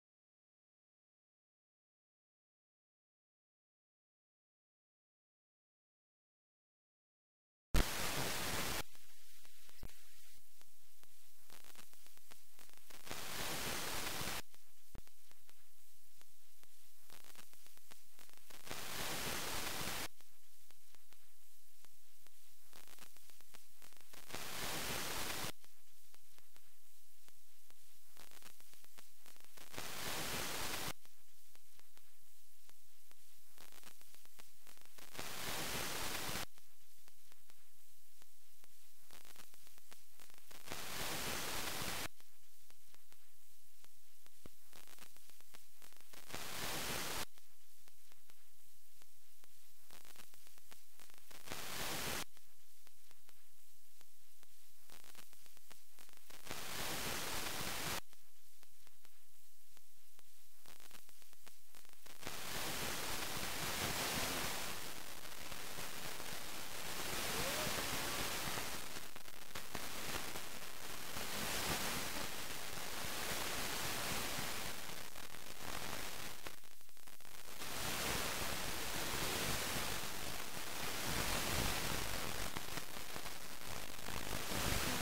FM DX Es- 130623 1552UTC- 97.7 - Vida FM (E) Vitoria=Gasteiz/Cruz de Zaldiaran(PVA-VI) 0.5kW! 1596km
97.7 – AB98, blank stereo carrier

97.7 s E VIDA FM: AB98 Vida FM Vitoria=Gasteiz/Cruz de Zaldiaran (PVA-VI)
0.5kW! 1596km 255°